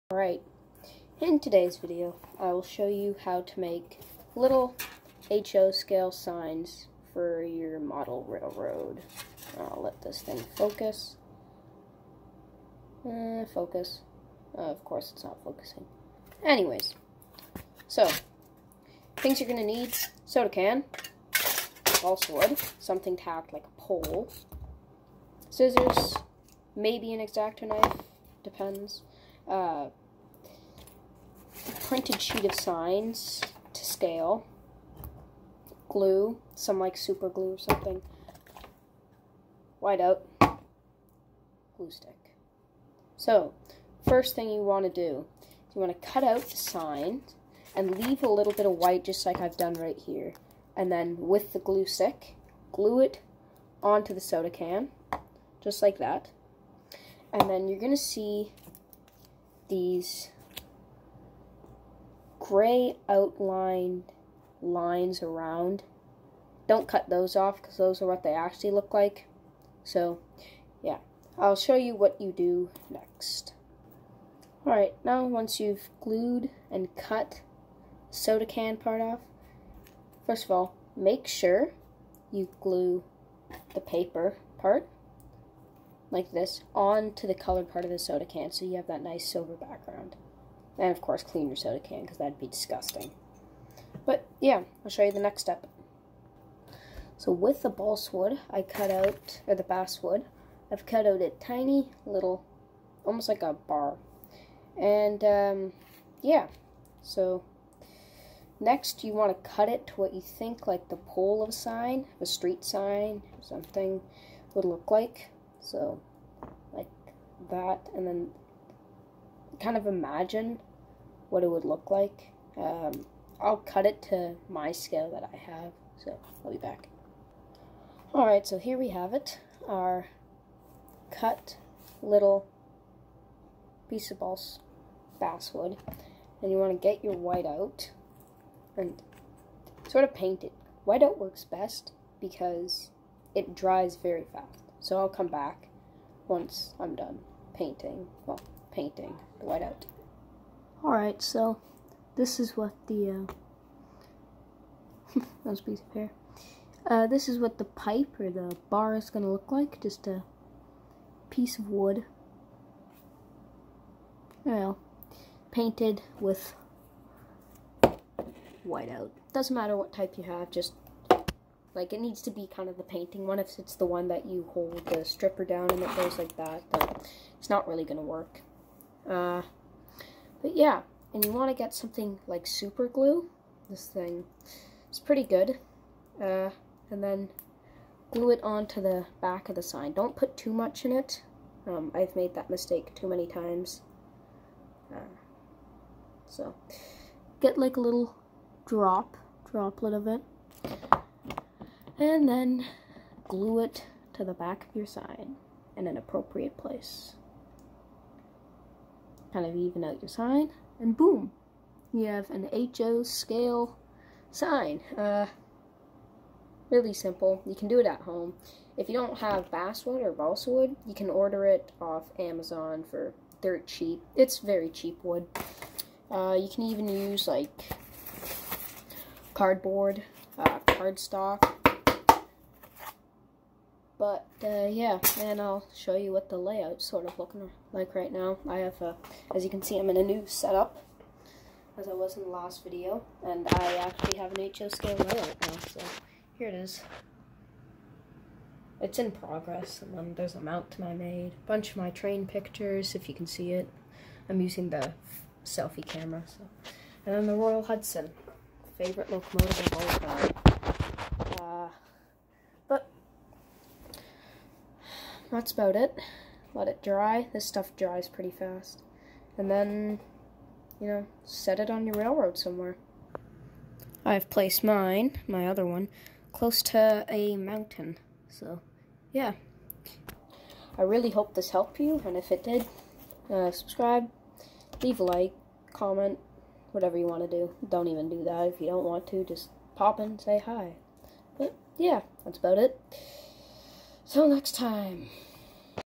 How to make street signs for model railroad
This is my own way of making street sign. They're are different ways in doing it but this is my way. If you enjoyed and if this helped please make sure to give the video a like if can.